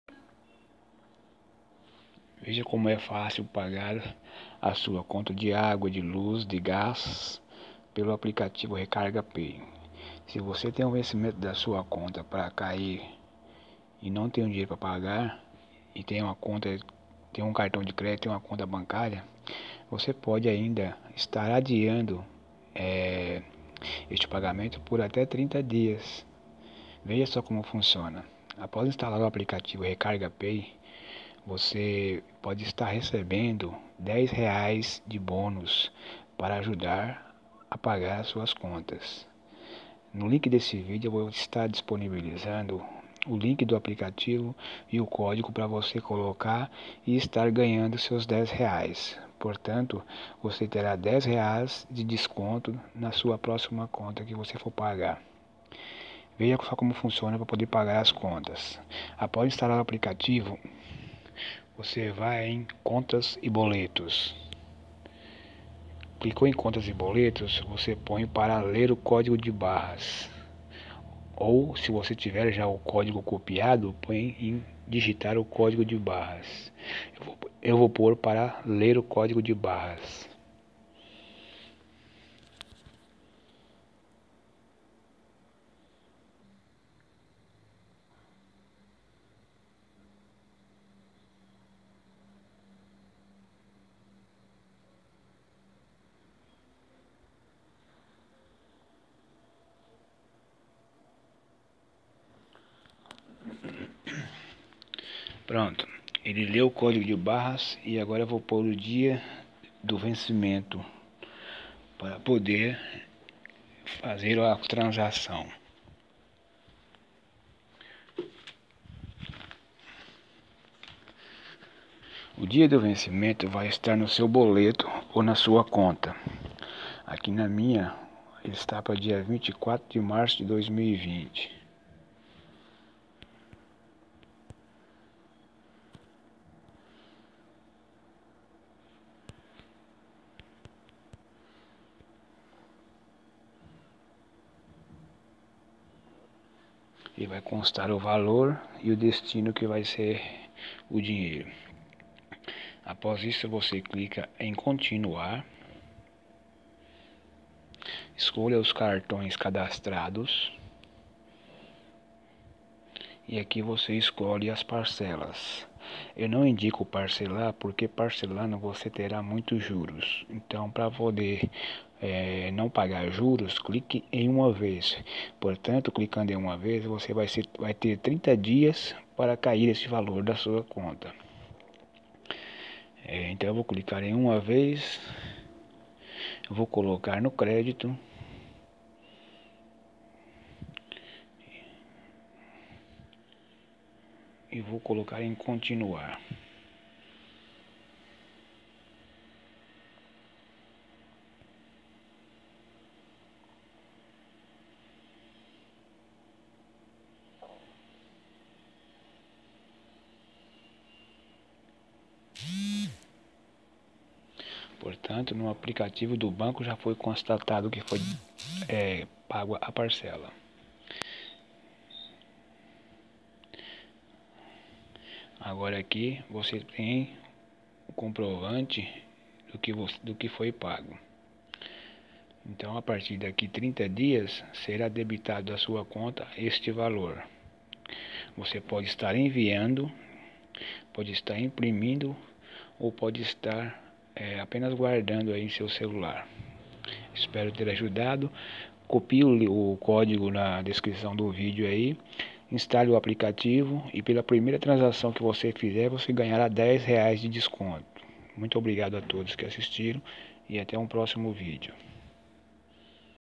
Pagando contas e Boletos sem sair de casa rápido e facil
Como pagar contas - boletos-carregar cartão de transporte -celular e outros
Ganhe 10 reais na primeira transação com o código
xc23085566
E economize muuuiiitooo tempo e ainda você pode ganhar muitos bônus com este app
Eu já uso a mais de um ano e sempre pago minhas contas por ele